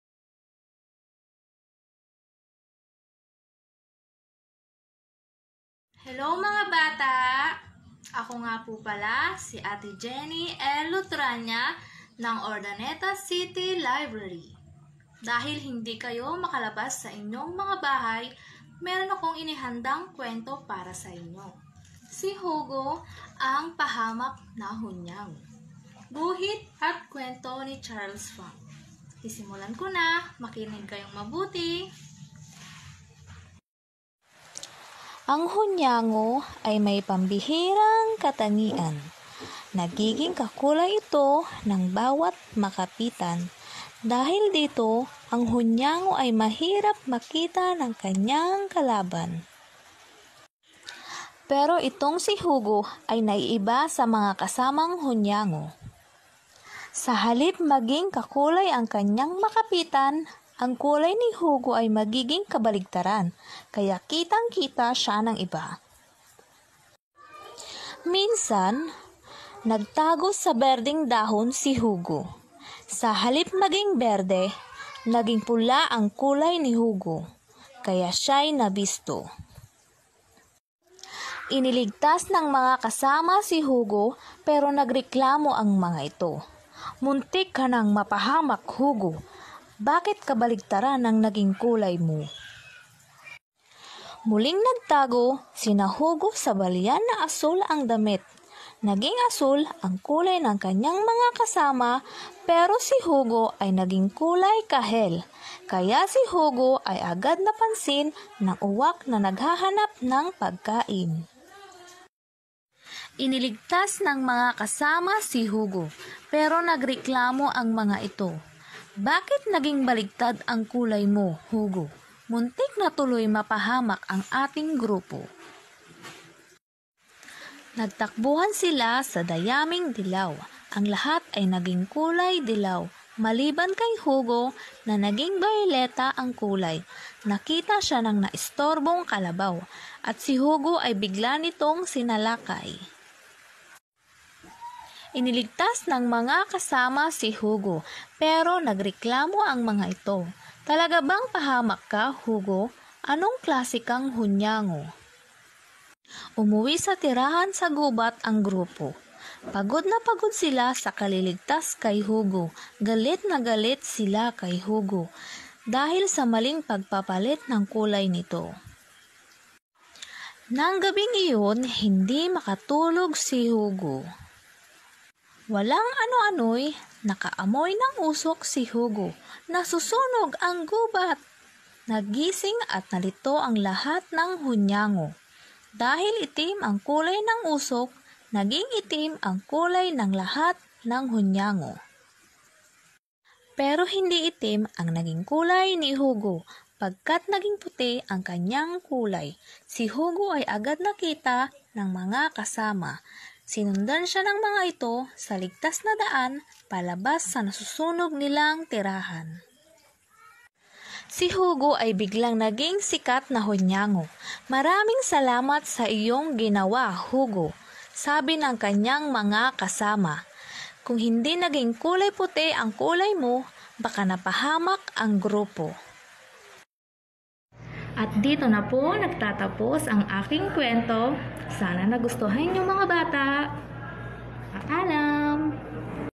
"Si HUGO ang Pahamak na Hunyango"
Ang kuwentong ito ay nagbibigay galak, nakapagtuturo at nakapagbibigay ng liwanag para sa inspirasyon sa buhay. Pakingan natin and ating kwentista ngayong linngo. Si ate JENNY LUTRANIA,  kawani ng Urdaneta City Library.